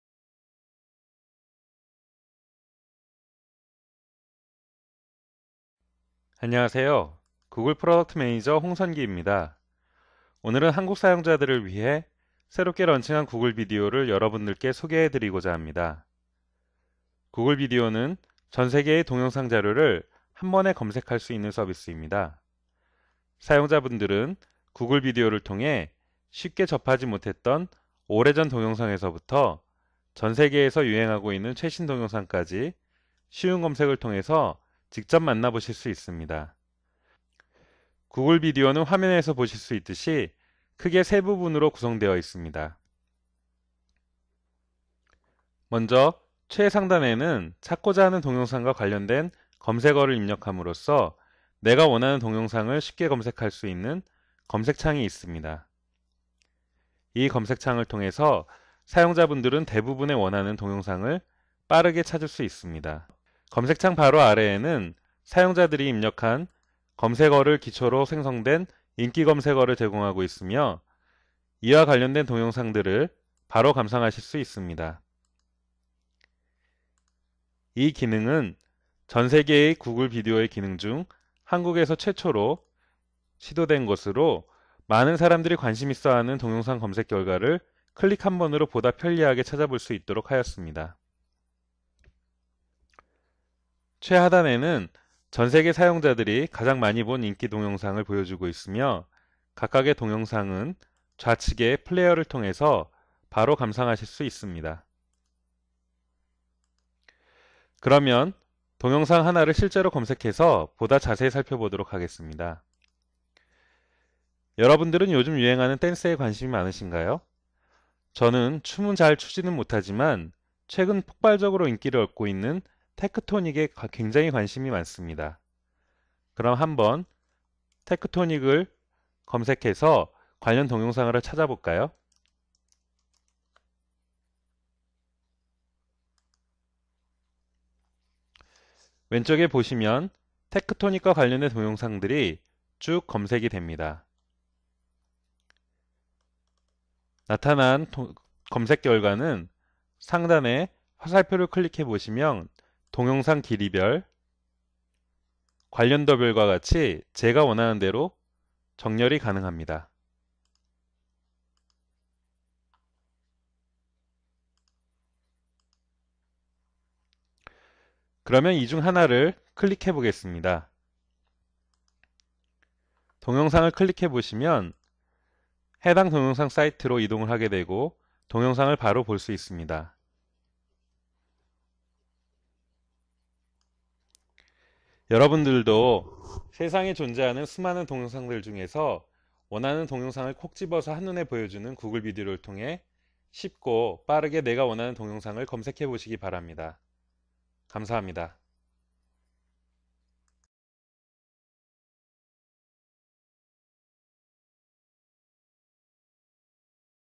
구글 비디오로 전세계 동영상을 검색해 보세요!
동영상 검색은 "구글비디오"에서 하면되고~
동영상 업로드는 "유튜브"에서 하면되고~

구글 비디오는 전세계의 동영상 자료를 한 번에 검색할 수 있는 서비스입니다. 사용자 분들은 구글 비디오를 통해 쉽게 접하지 못 했던 오래 전 동영상에서부터 전 세계에서 유행하고 있는 최신 동영상까지, 쉬운 검색을 통해서 직접 만나보실 수 있습니다.

여러분들도 세상에 존재하는 수많은 동영상들 중에서 원하는 동영상을 콕 찝어서 한 눈에 보여주는 구글 비디오를 통해서, 쉽고 빠르게 내가 원하는 동영상을 검색해 보시기 바랍니다.

아래 동영상을 통해 구글 비디오 검색서비스에 대해 좀더 자세히 만나보실 수 있습니다.